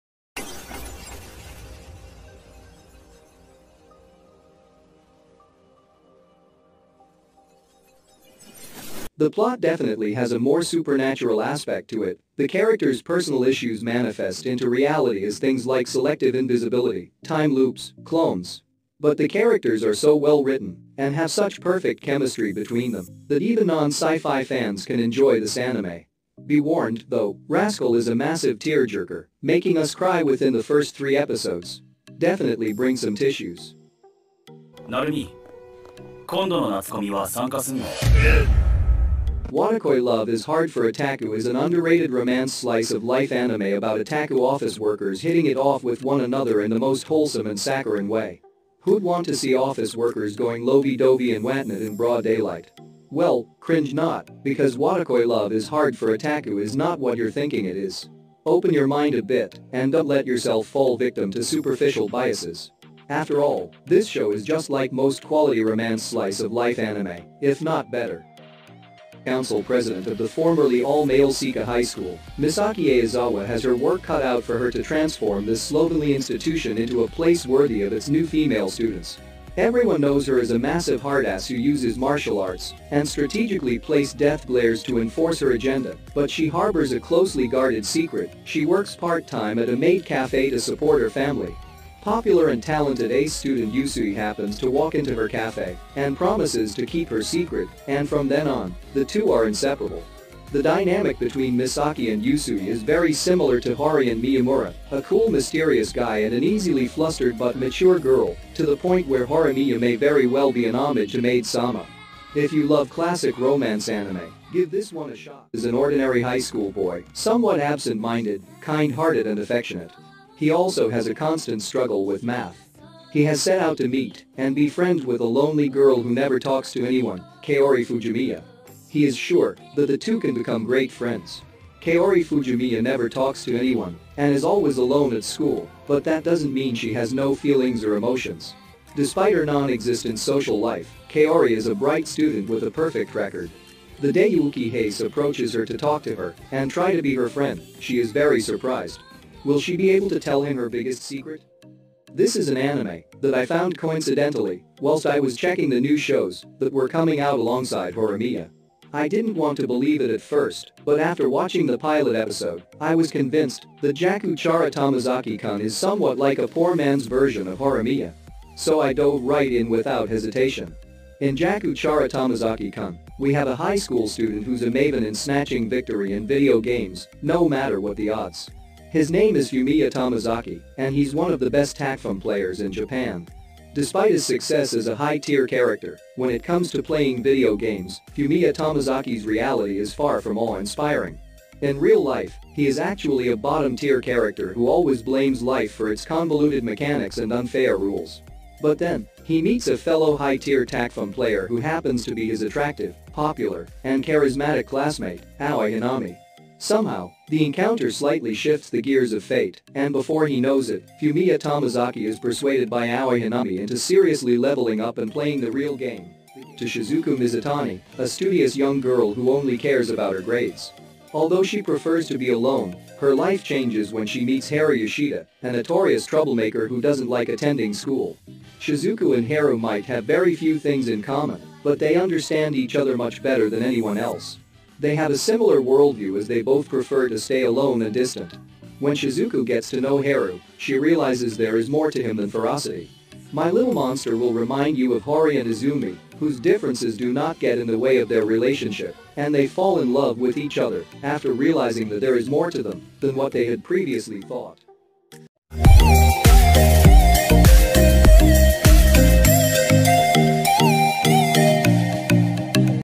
6 romantic comedy ❤️ anime part 3🔥🔥new
Hey guys as you all know, in this series I am gonna show 6 romantic comedy anime, I hope you will like the list

I hope you guys will like the video, if you do then please share this with your friends ☺️